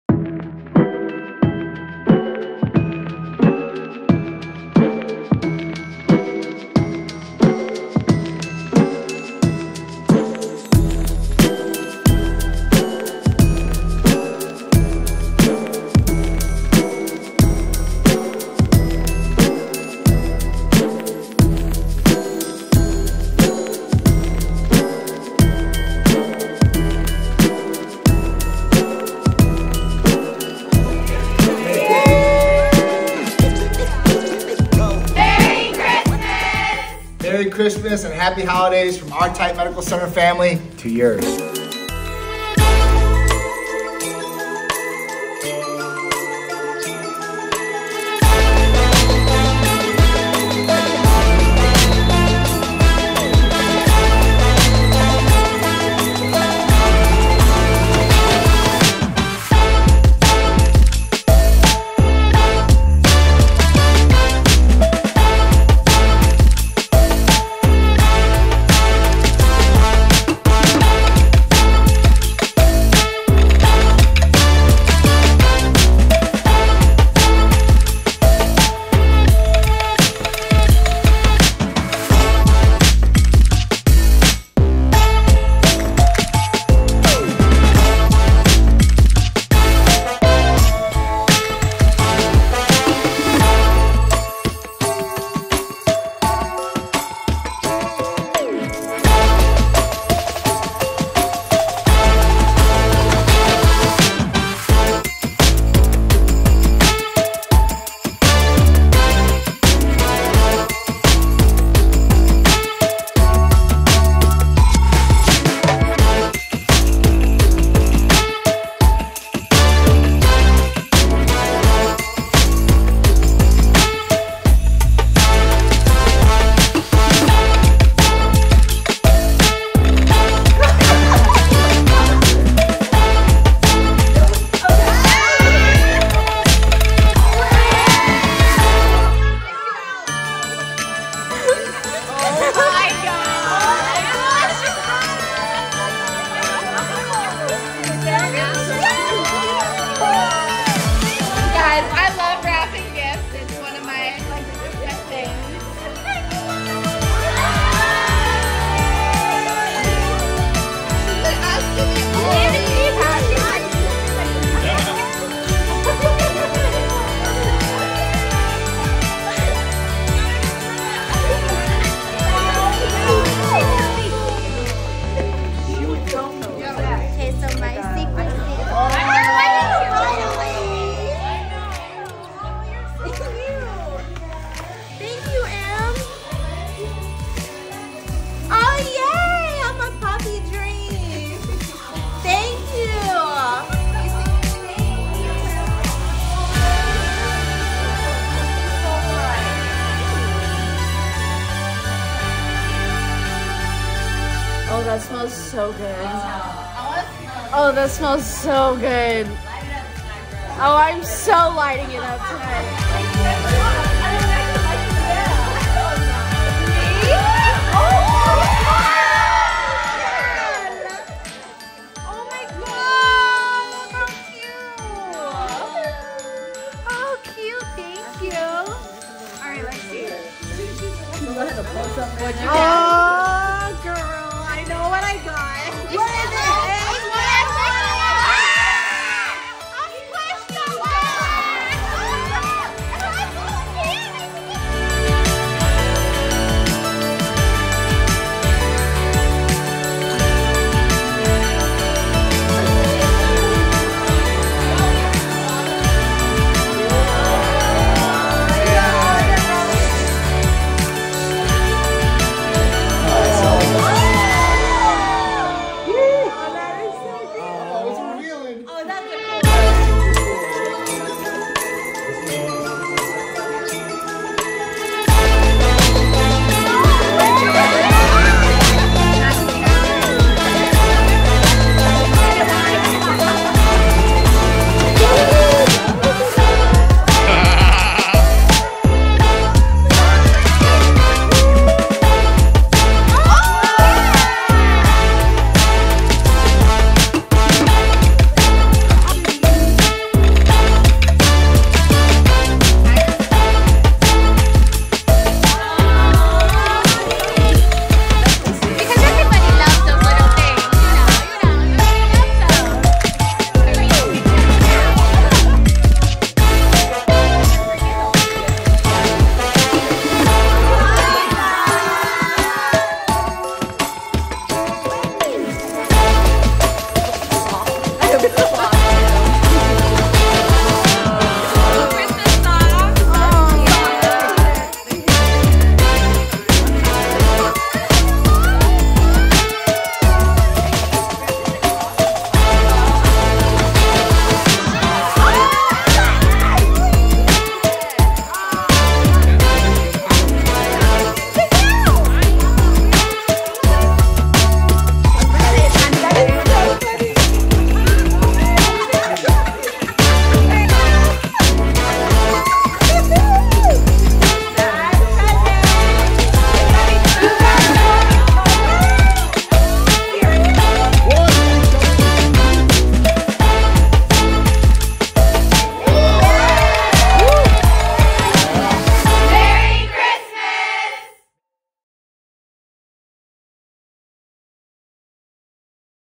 Christmas Office Party at Titan Medical Center 2023!
We had our 2023 Christmas Party at the Titan headquarters and it was awesome! Everyone got some great gifts and had a great time.
Happy Holidays to everyone from our Titan Family to yours!